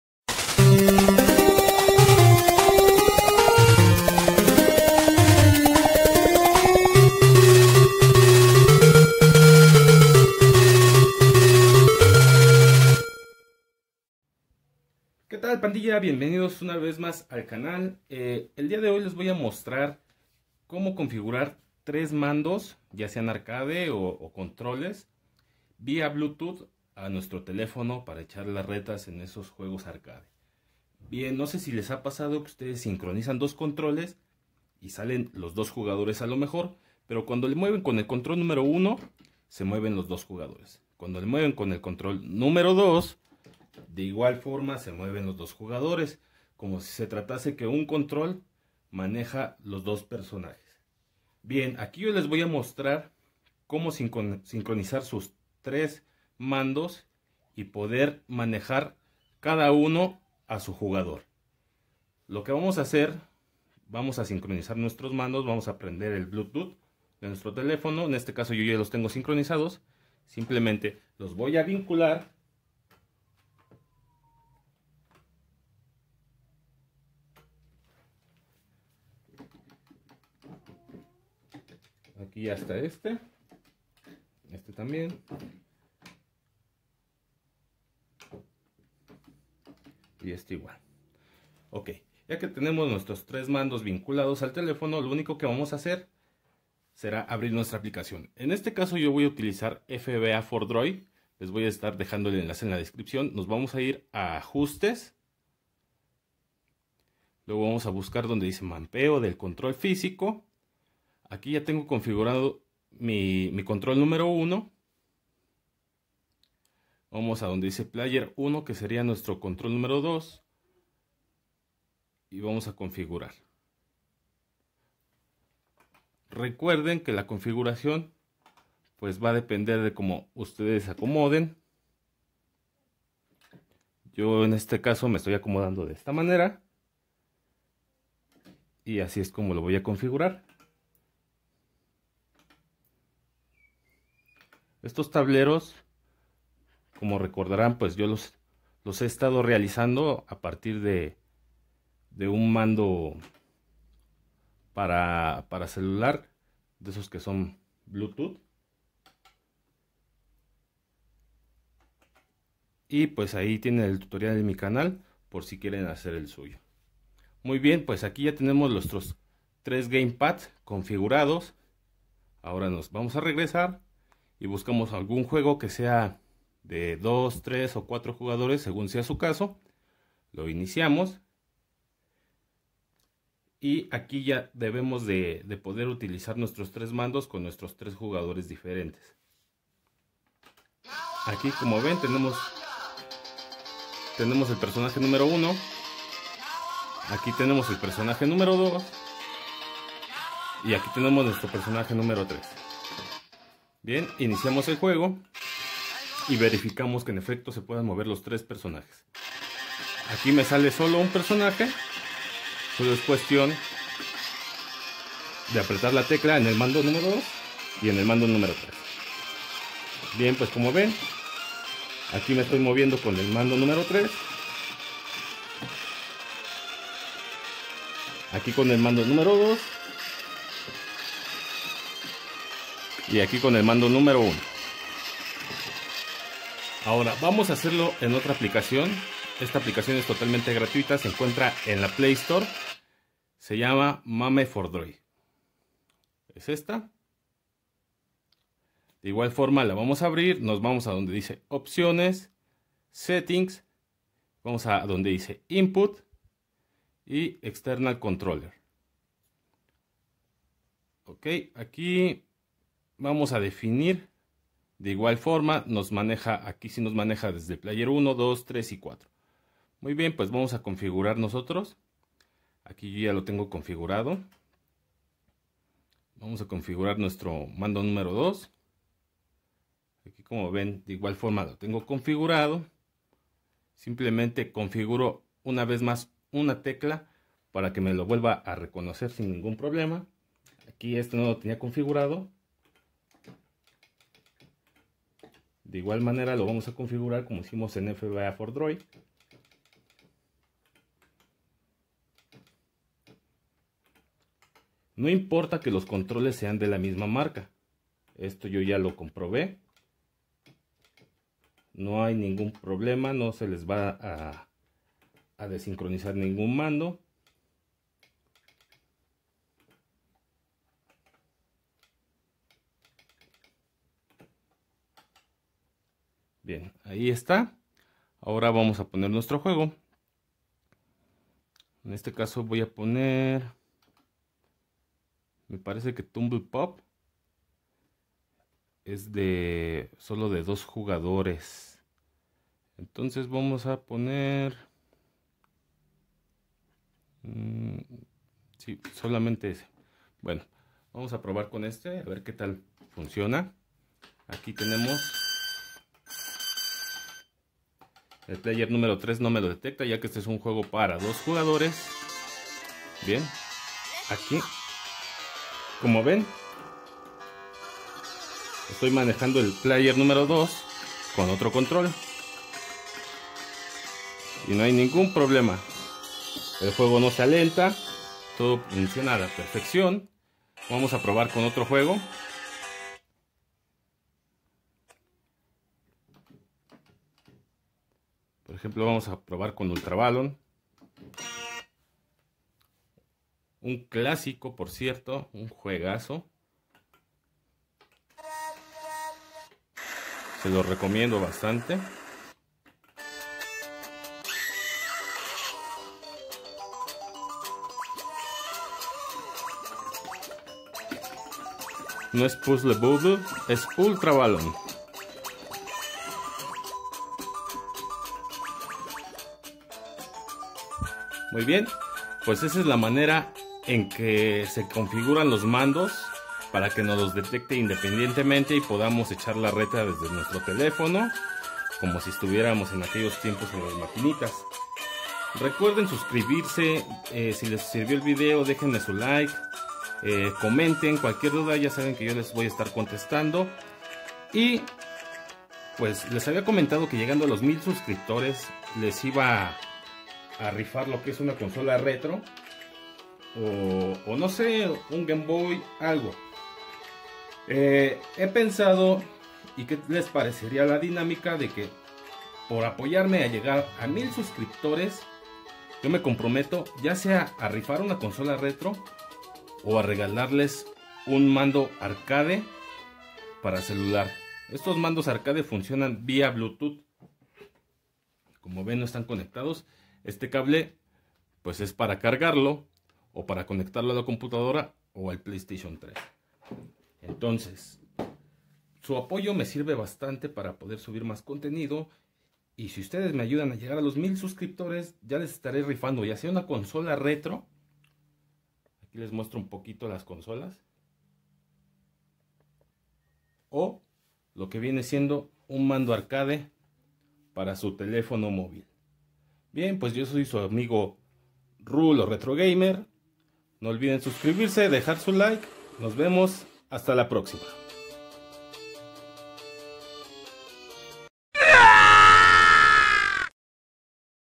Como conectar 2 mandos o mas al celular por Bluetooth!!!🎮🎮🎮🎮📱😎
Hola pandilla retro gamer pues como ven aqui les muestro la manera de configurar sus controles para poder jugar en el celular en modo multijugador, recuerden q a toda esa pandilla de México q me apoye a llegar a los 1000 suscriptores voy a rifar una consola retro o un tablero en agradecimiento, cualquier duda dejenmela en los comentarios y con gusto les contesto!!!
S.U.S.C.R.I.B.E.T.E ! ! !

Link de los Emuladores la contraseña es:
ruloretrogamer